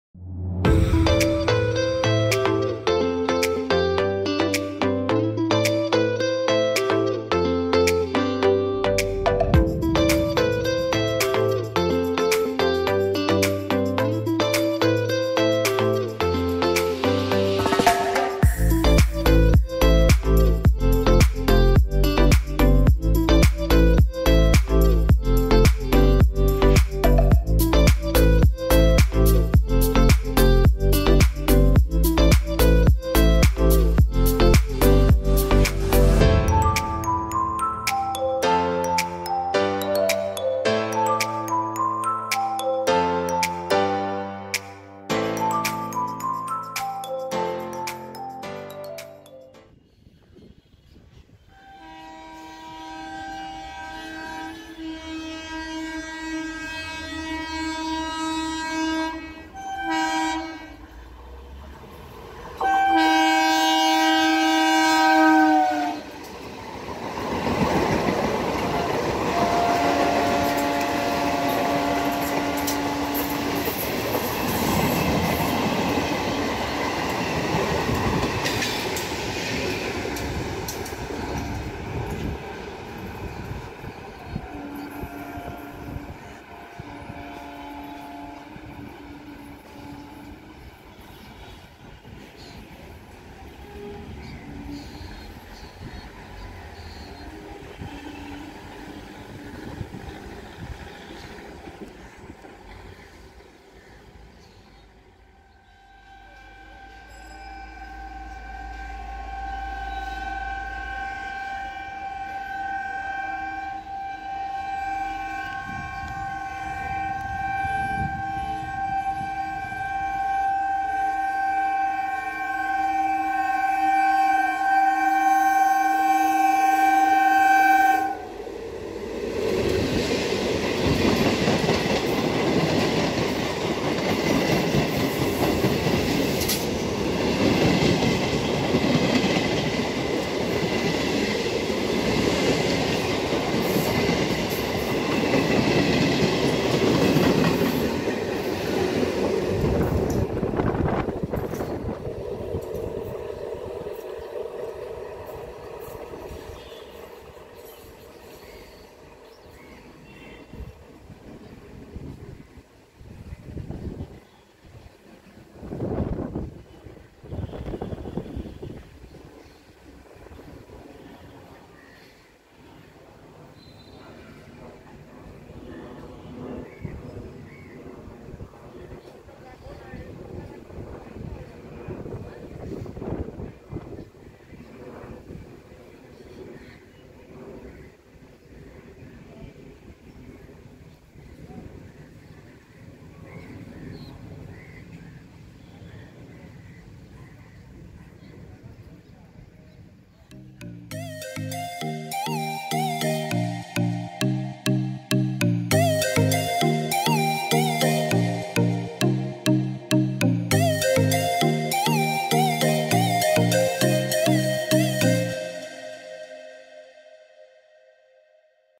|[2 in 1]| Longest Distance Galloping Staff Special EMU Train Compilation of Sealdah South Section.
Location